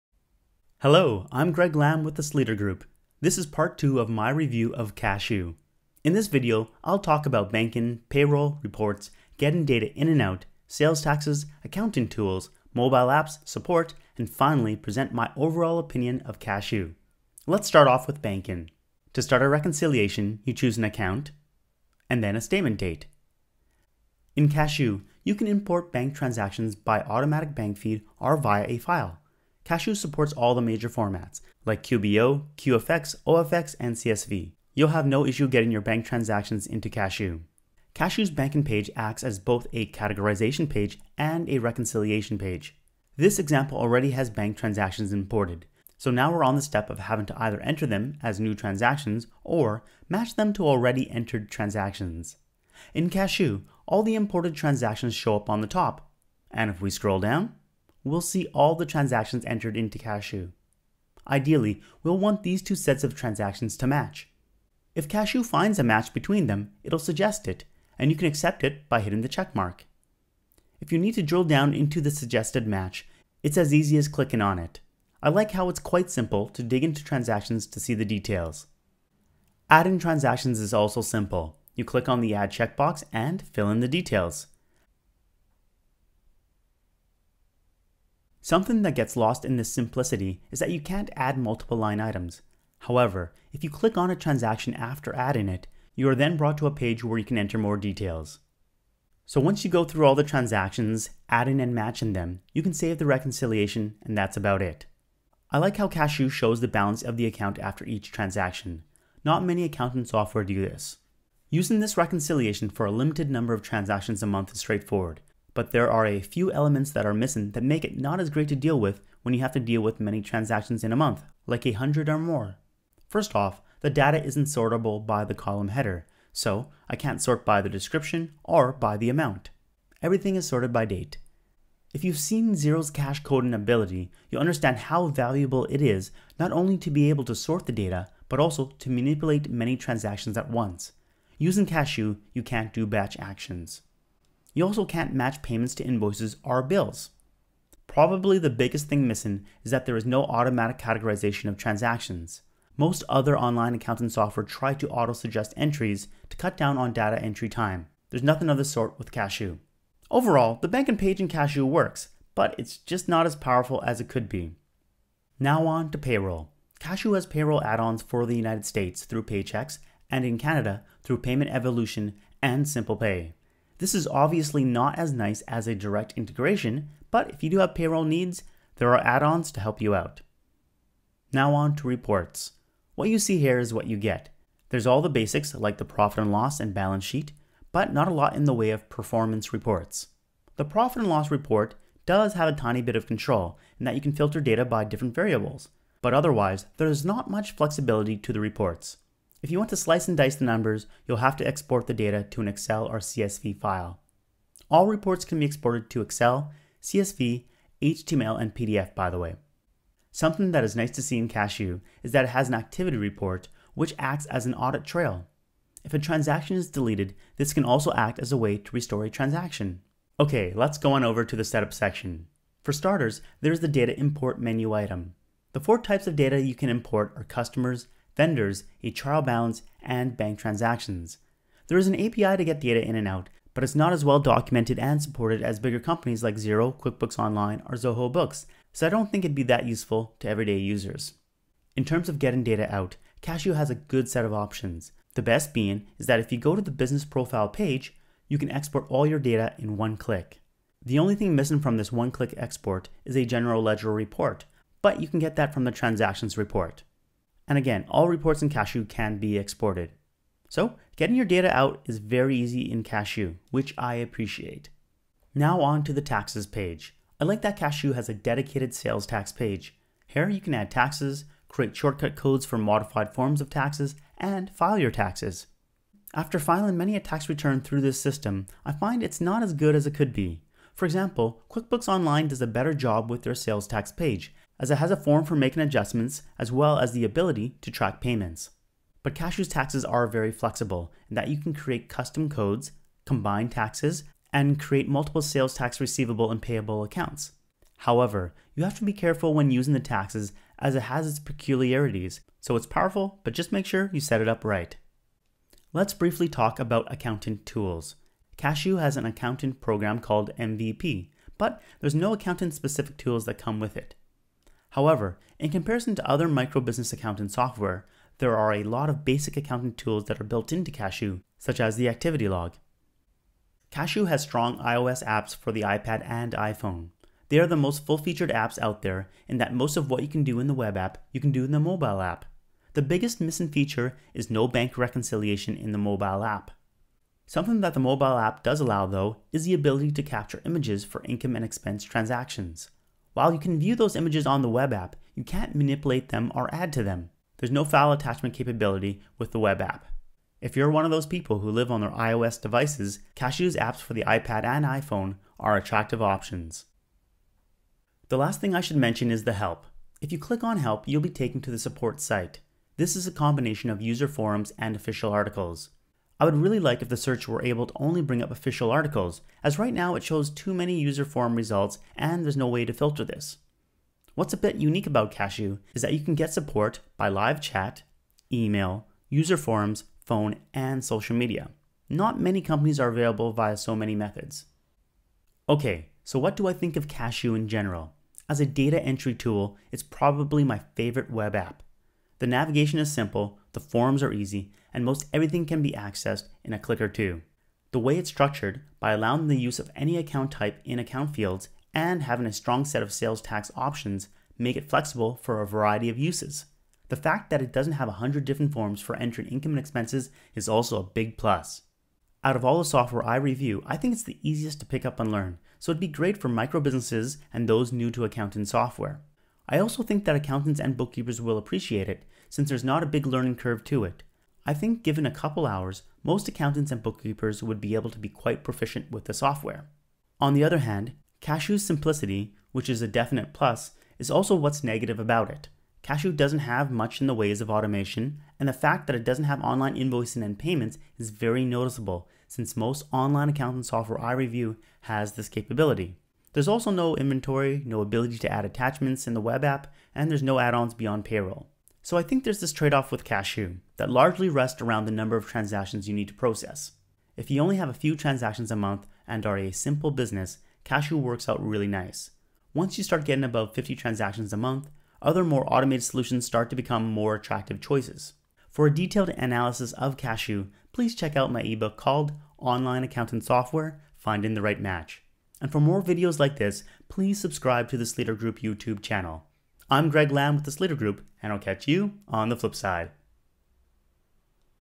Kashoo Accounting Software Review / Tutorial - Part 2
Greg Lam from the Sleeter Group reviews Kashoo accounting software. In part 2 of this review / tutorial, he'll discuss banking, payroll, reports, getting data in and out, sales taxes, accountant tools, mobile apps, and support. He'll also present his overall opinion of Kashoo.